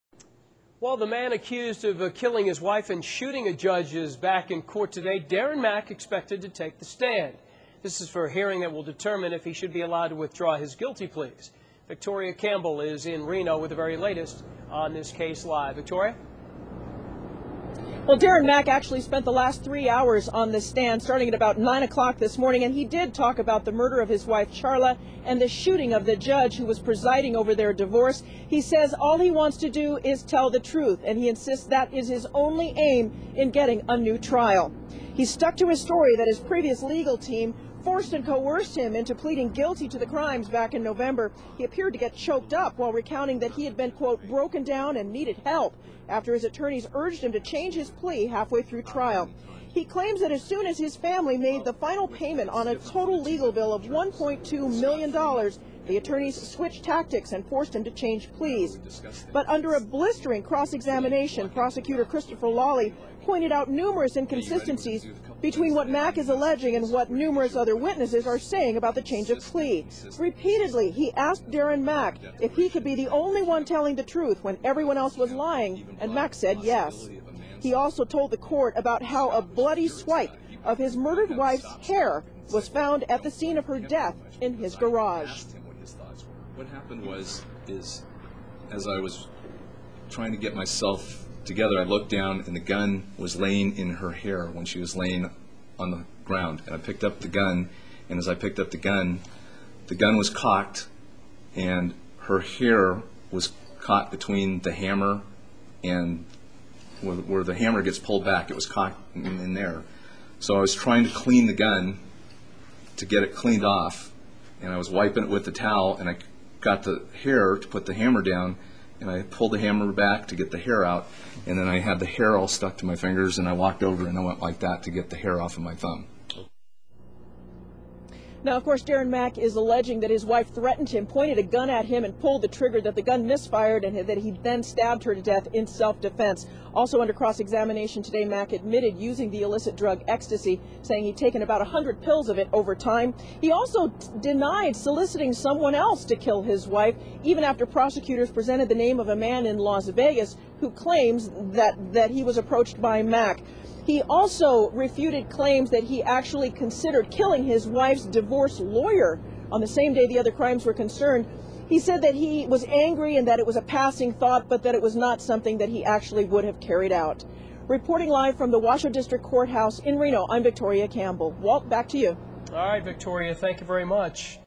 Accused Reno Killer Expected To Testify
Darrin Mack was expected to take the stand during a hearing to determine if he should be allowed to withdraw his guilty pleas.